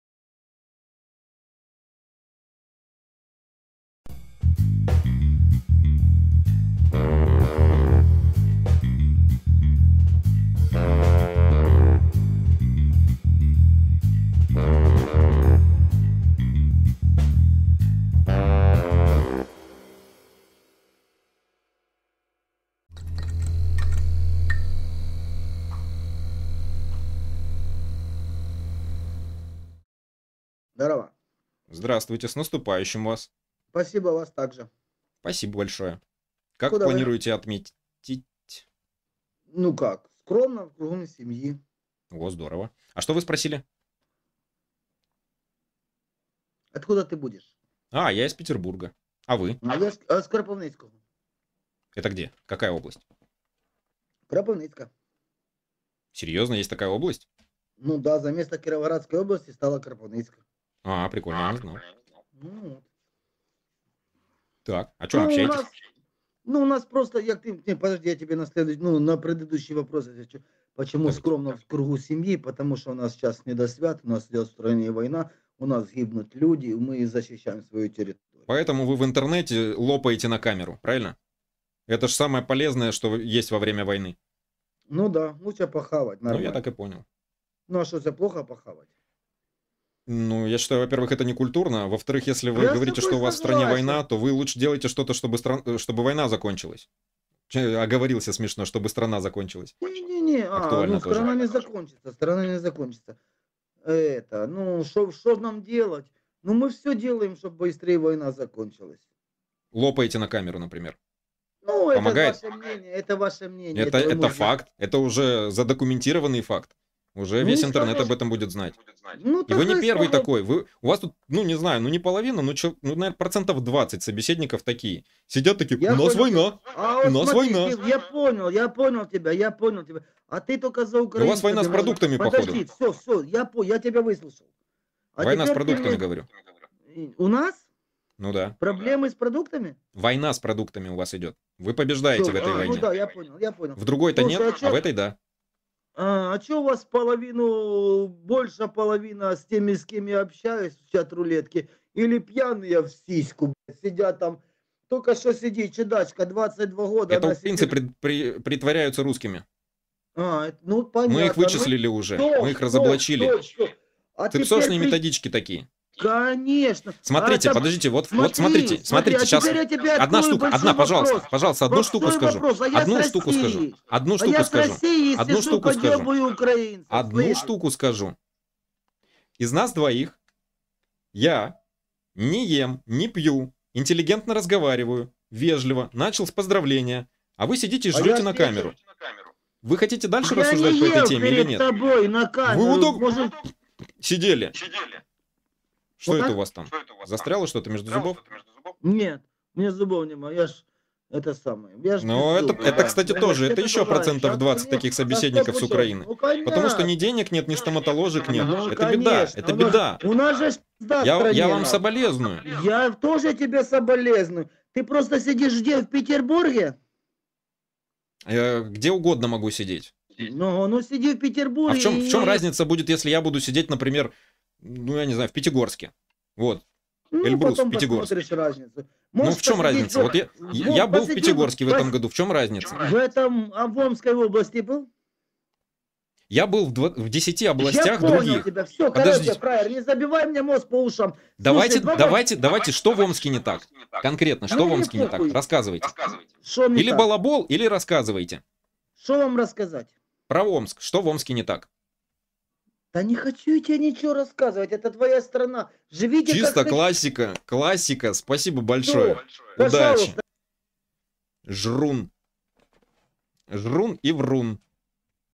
У них всё так, а вот в Омске — не так 🤷‍♂️ | Чат-рулетка с Украиной
О войне, в которой они побеждают, и об Омске, в котором «что-то не так».